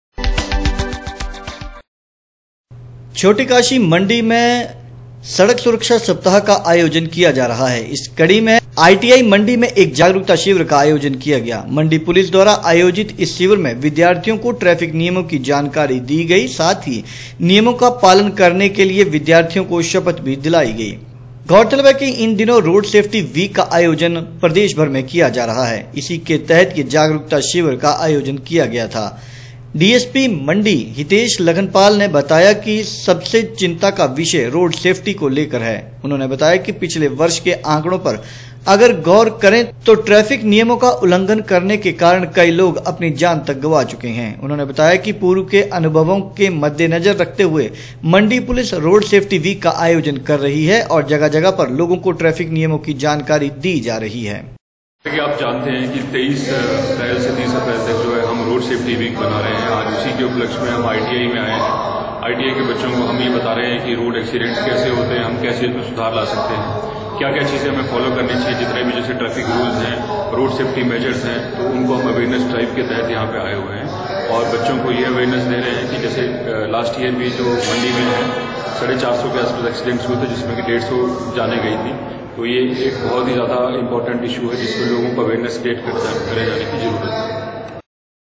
छोटीकाशी मंडी में वर्तमान समय सड़क सुरक्षा सप्ताह का आयोजन किया
छोटीकाशी मंडी में वर्तमान समय सड़क सुरक्षा सप्ताह का आयोजन किया जा रहा है इसी कड़ी में वीरवार को आई टी आई छोटीकाशी मंडी में वर्तमान समय सड़क सुरक्षा सप्ताह का आयोजन किया जा रहा है इसी कड़ी में वीरवार को आई टी आई मंडी में एक जागरूकता शिविर का आयोजन किया गया मंडी पुलिस द्वारा आयोजित इस शिविर में विद्यार्थियों को ट्रैफिक नियमों की जानकारी दी गई